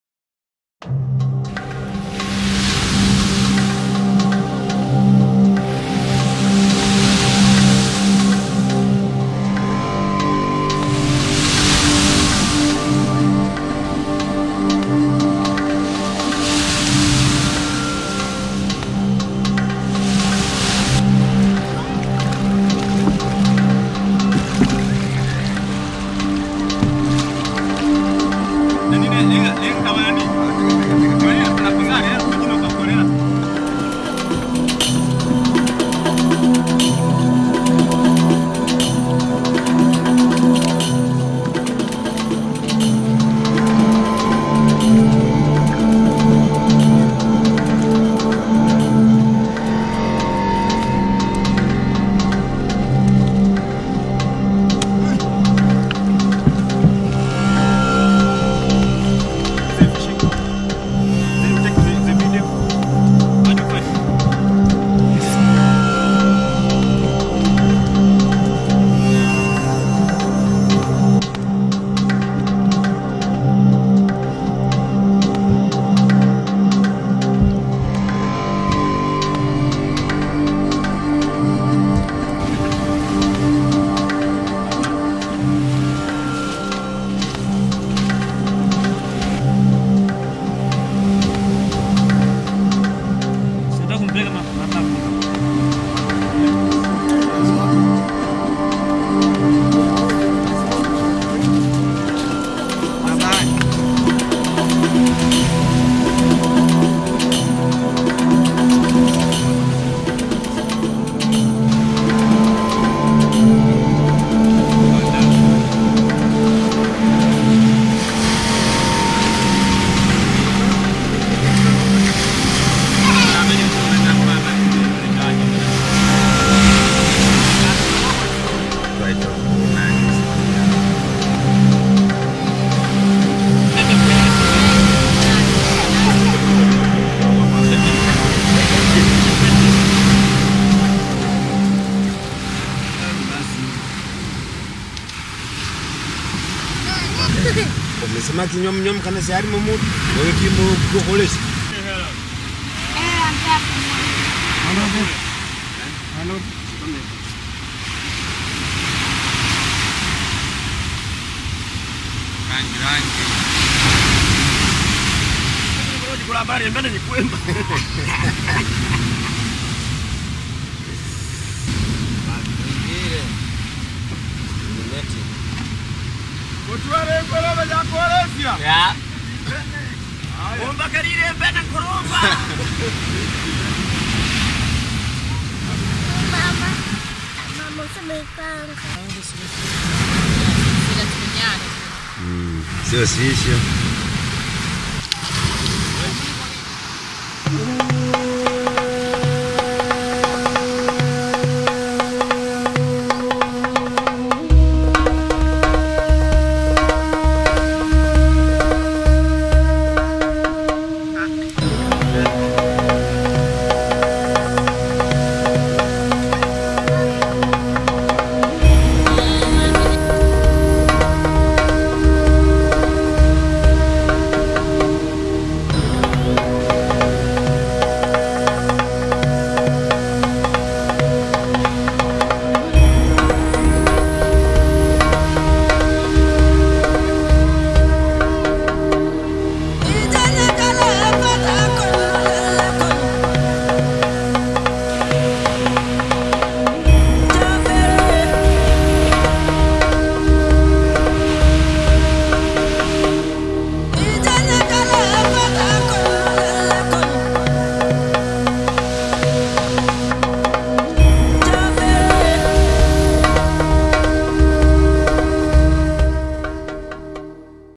Greetings from lake malawi.
I visited Lake Malawi several times,most in the north of the lake on Tanzania's coast. I went there once with some fishermen in their cano who caught the fish with their nets, mainly to sell at the local market. But I was still surprised what came out of the nets.